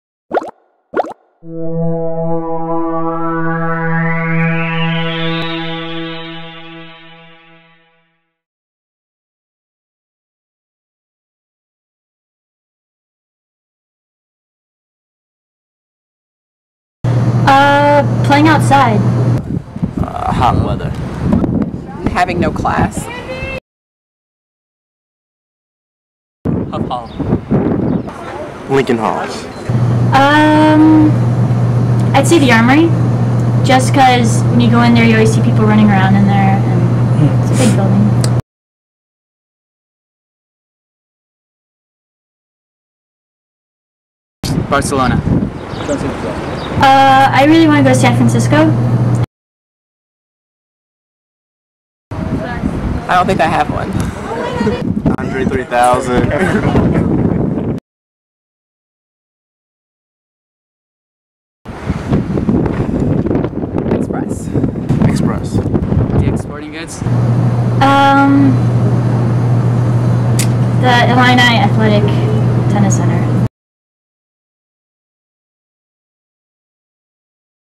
Illini Athletes Sound Off 4/18
Illini athletes Julian Smith, Morolake Akinosun and the Kopinsky twins answer our questions about life outside of sports. Questions include their favorite building to have classes in and which celebrities they think they look like.